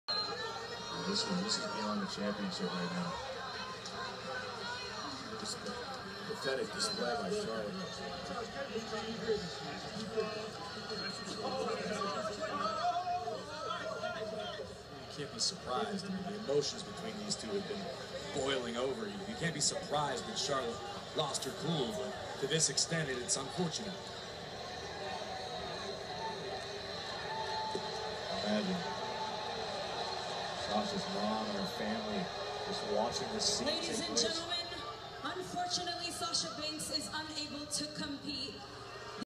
WWE Fake EMT fail
EMT wraps Sasha banks neck with camera cables